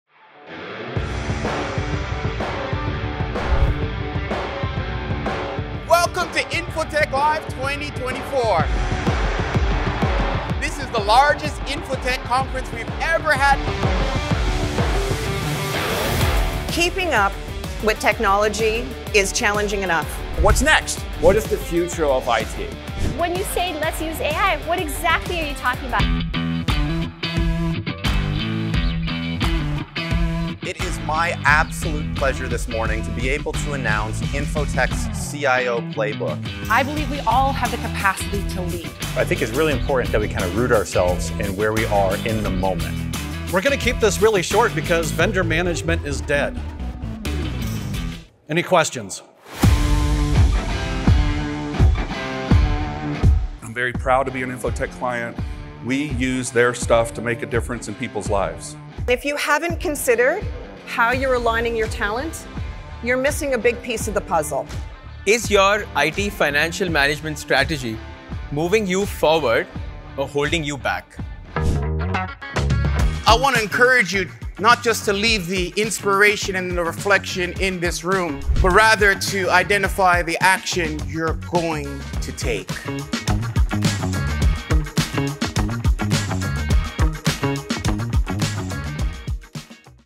Inside the World’s Premier IT Leadership Conference | InfoTechLIVE 2024 Highlights 🚀💻
This year, Info-Tech LIVE brought together over 2,000 IT executives and facilitated over 700 analyst discussions to explore the future of technology and innovation. The event focused on providing actionable tools, knowledge, and insights that IT leaders need to thrive in an exponentially complex and fast-paced technological world.

Couldn’t make it? We’ve got you covered!